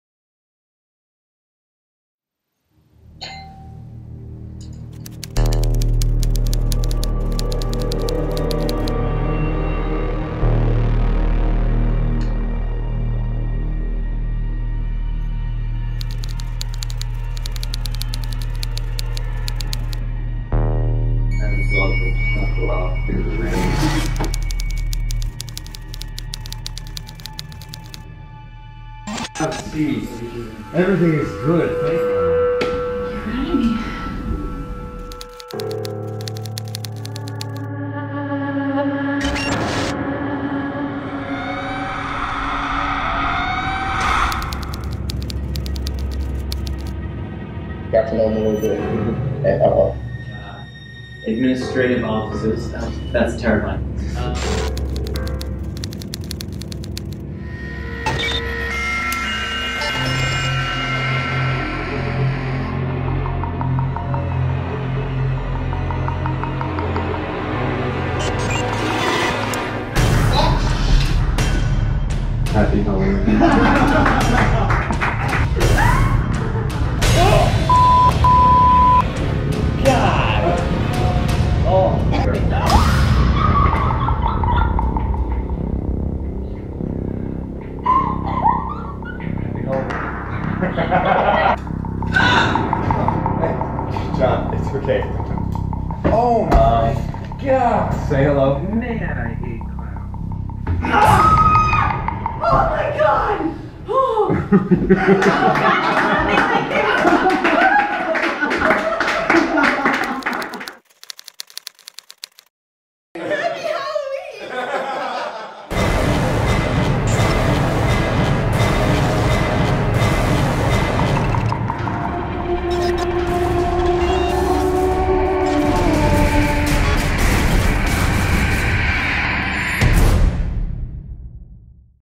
The world's scariest job interview
CareerBuilder decided to have fun with a few job seekers this Halloween. If you thought job interviews were scary before, try adding a few clowns into the mix.